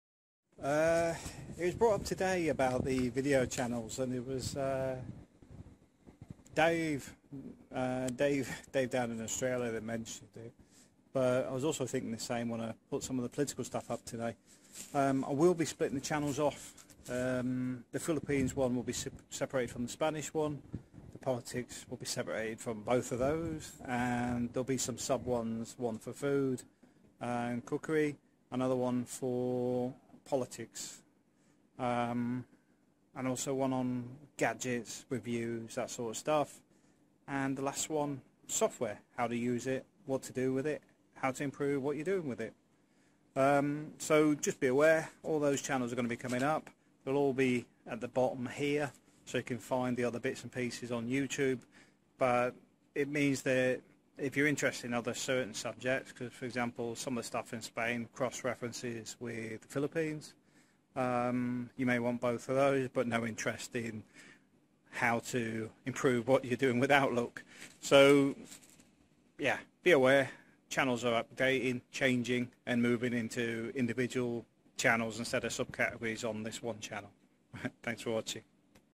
This Channel is splitting some of its subjects
Dave mentioned that the subjects are a bit all over the place. As well as myself thinking the same when I decided to start adding politics, so what I have decided to do is split the sub-categories into specific channels. This means you will only get updated with relative information. E.g. if your just after the Philippines stuff stick to this channel. If you want to know about Spain, Video editing or politics they are all going onto their own channels.